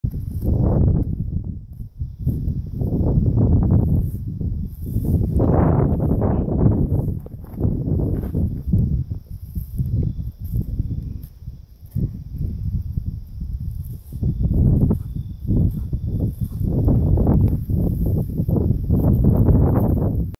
Flint knapped knife skinning a Pronghorn part 1
This is how I skinned a Pronghorn,  with an obsidian knife I knapped, Handle is elk antler.
Flintknapping,  awesome knife.